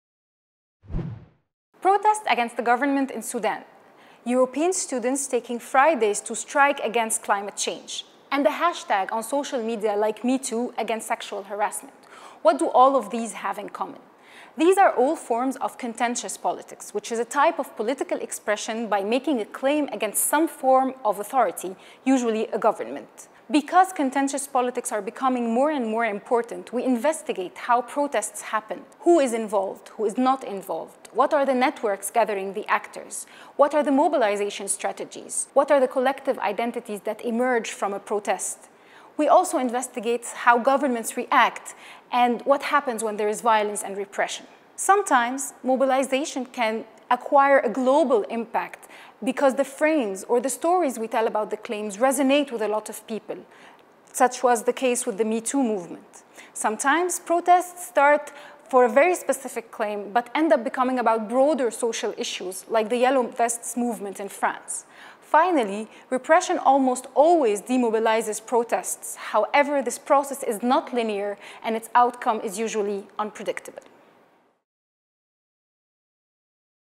Contentious politics | Farah Ramzy - Schuman Short #45
Protests against the government in Sudan; students striking for climate... What do these events have in common? Max Weber Fellow Farah Ramzy explains 'Contentious politics'.